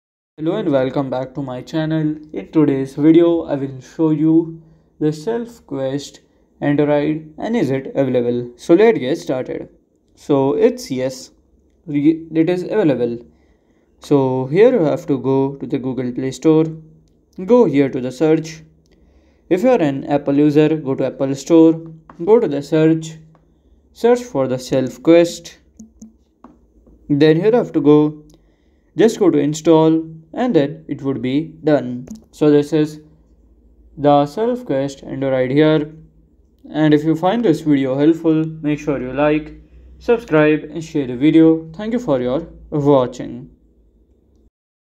✅ SELFQUEST ANDROID - is it available on Android? (Full Guide)
Curious whether SelfQuest is available on Android devices? This guide provides the latest information on SelfQuest's availability on the Google Play Store and how you can access it.

Key Learning Points:
• Current status of SelfQuest on Android
• Tips for Android users awaiting SelfQuest